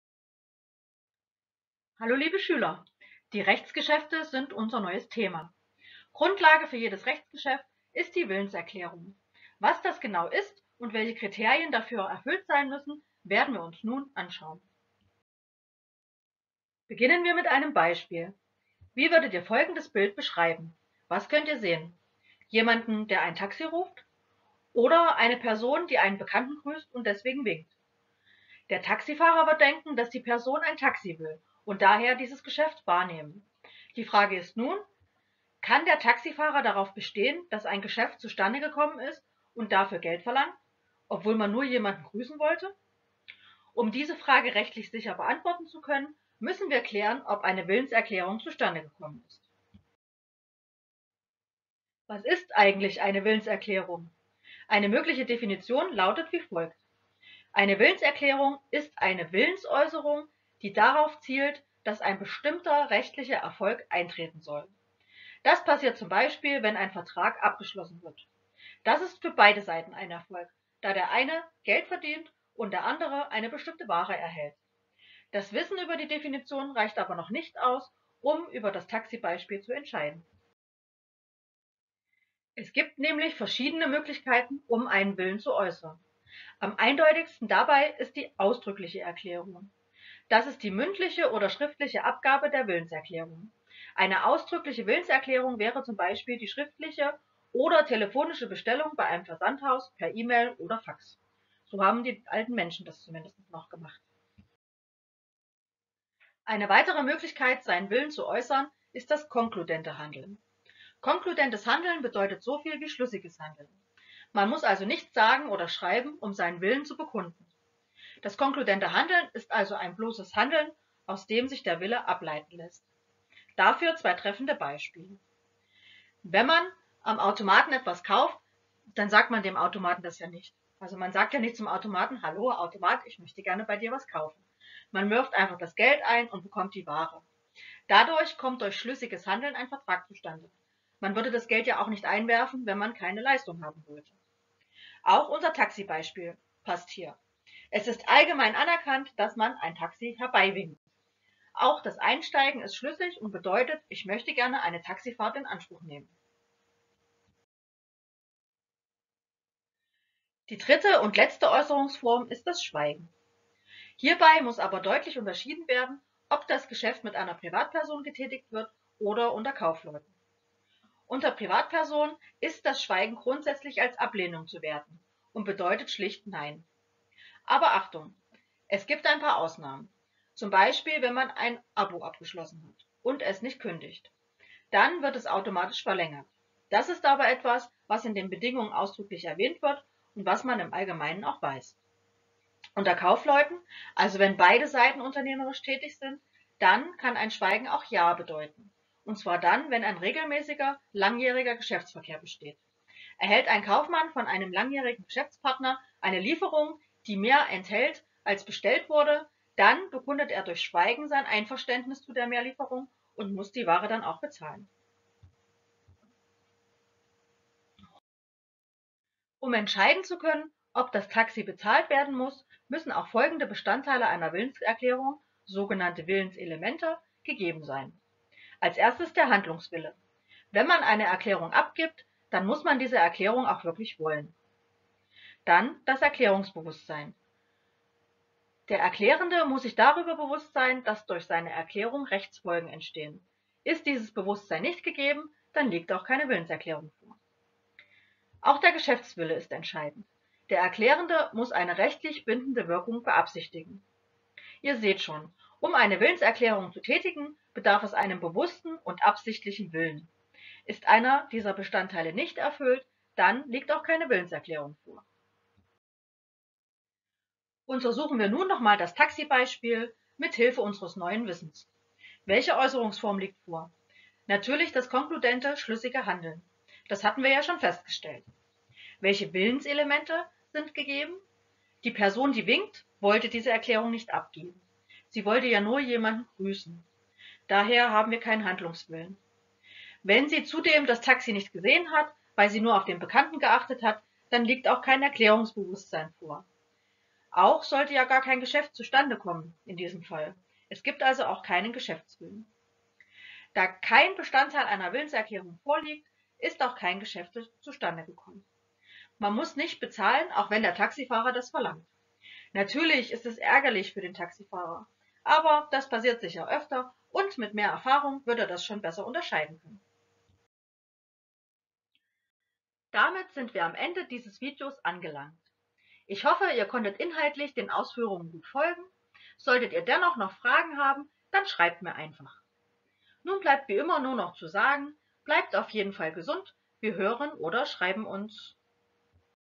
BWL - Willenserklärung in Rechtsgeschäften
Liebe Schüler,
In diesem Video geht es um die Willenserklärung in Rechtsgeschäften. Ihr erfahrt etwas über die Definition, die Äußerungsform einer Willenserklärung und der notwendigen Bestandteile einer Willenserklärung.

Viel Spaß beim Lernen und bleibt gesund 🍀

Mrs K 👩🏼‍🏫